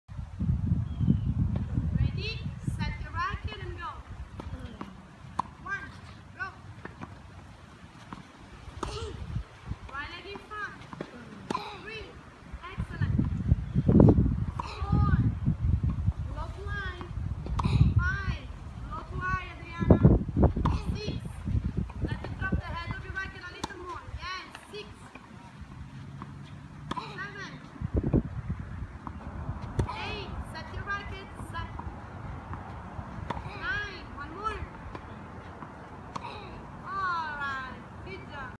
Adriana at Macci Academy - 3
6 years old, green dot balls, more backhands. Thank you, Sylvia, for the excellent coaching.